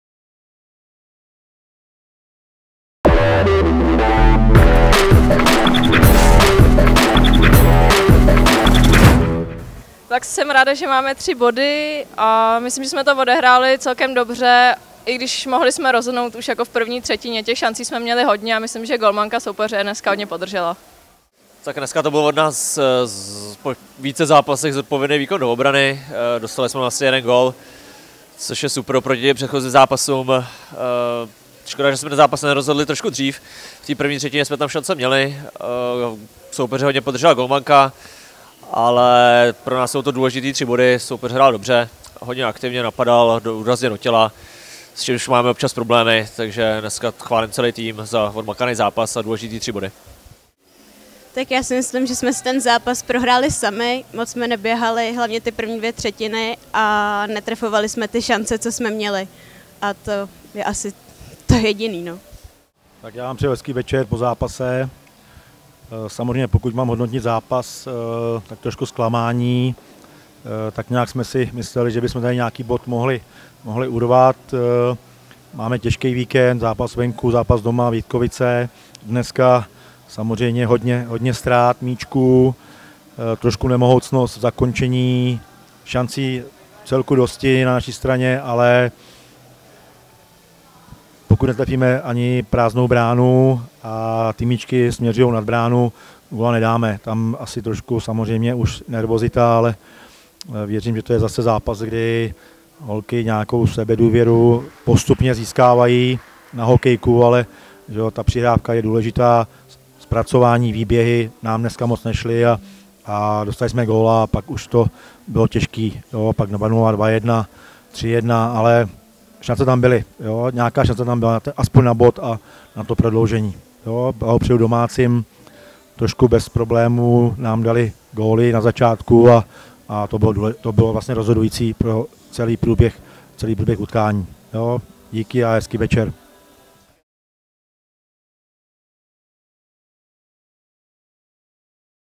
OHLASY | Panthers Praha - Fl. akademie MB (27.02.2021, 11. kolo)
Ohlasy po utkání 11. kola Extraligy žen mezi Panthers Praha a Florbalovou akademií MB, které za domácí zhodnotila Kristýna Horňáková a Pavel Skazko, za hosty pak Eliška Horáková a Radek Novák.